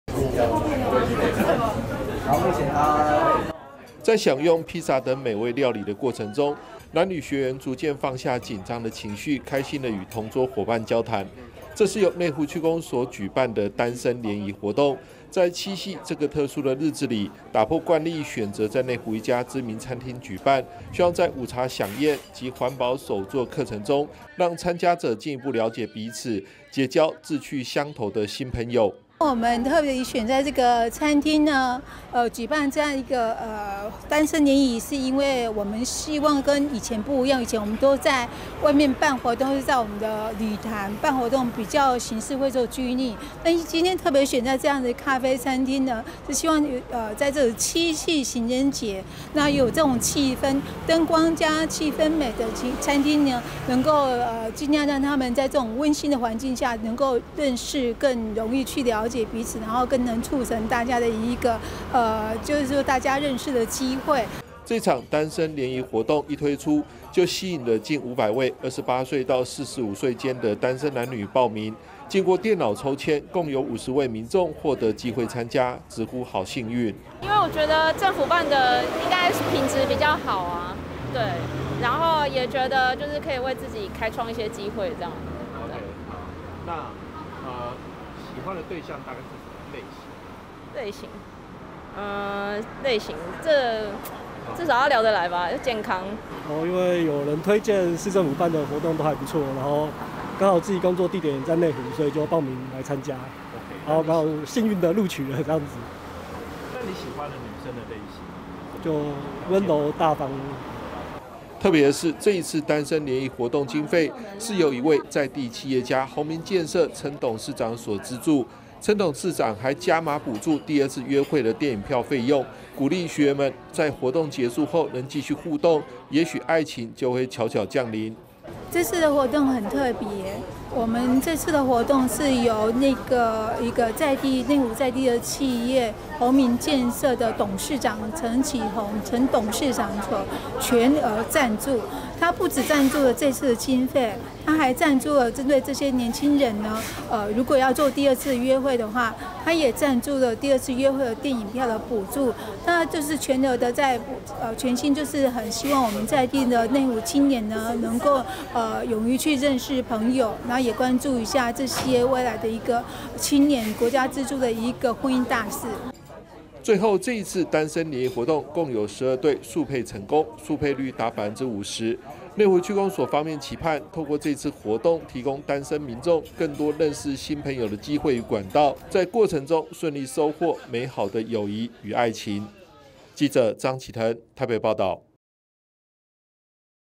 七夕 單身聯誼|內湖 企業家|餐廳 美食|約會 電影票|2024 中嘉新聞|在地企業家贊助 內湖七夕辦單身聯誼 二次約會補助電影票
企業家贊助內湖區公所舉辦單身聯誼 還加碼給二次約會者補助電影票經費

（中嘉新聞記者張啓騰/台北內湖區報導）
為了提供單身男女更多認識異性朋友的機會，在在地企業家宏明建設陳董事長熱情贊助下，內湖區公所在七夕這個特殊的日子選擇在一家知名餐廳舉辦「邂逅內湖 緣定七夕」單身聯誼活動，共有50位男女學員參加，而且在活動結束後如果進行第二次約會還可以獲得電影票補助，希望鼓勵學員們進一步了解彼此，也許愛情就會悄悄降臨。

在享用披薩等美味料理的過程中，男女學員逐漸放下緊張的情緒開心與同桌夥伴交談，這是由內湖區公所舉辦的單身聯誼活動，在七夕這個特殊的日子裡打破慣例選擇在內湖一家知名餐廳舉辦，希望在午茶饗宴及環保手作課程中，讓參加者進一步了解彼此，結交志趣相投的新朋友。

這場單身聯誼活動一推出就吸引了近五百位28歲到45歲間的單身男女報名，經過電腦抽籤，共有50位民眾獲得機會參加，直呼好幸運。

特別的是這次單身聯誼活動經費是由一位在地企業家宏明建設陳董事長所贊助，陳董事長還加碼補助第二次約會的電影票費用，鼓勵學員們在活動結束後能繼續互動，也許愛情就會悄悄降臨。

最後這次單身聯誼活動共有12對速配成功，速配率達50％，內湖區公所期盼透過這次活動提供單身民眾更多認識新朋友的機會與管道，在過程中順利收獲美好的友誼與愛情。